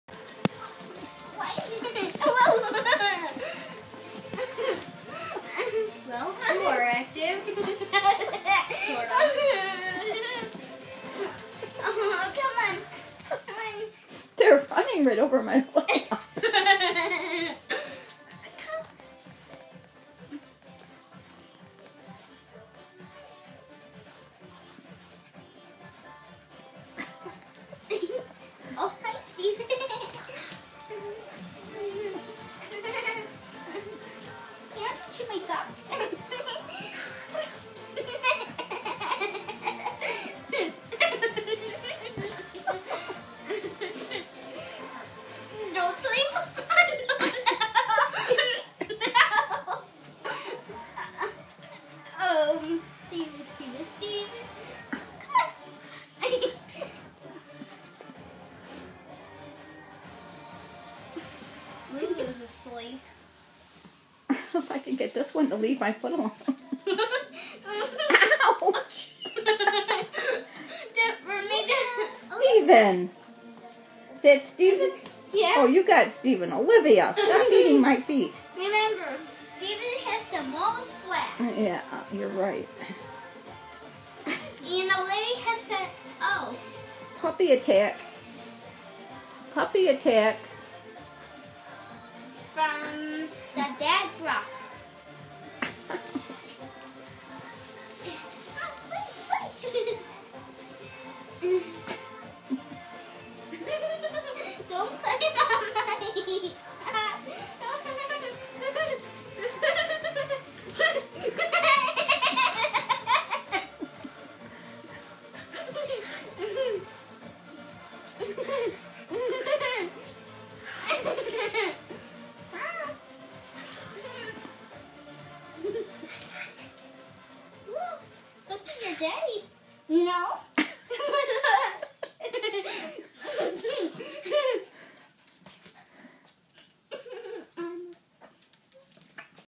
Daddy Day Care part one!
Daddy  showing his puppies how to play ball.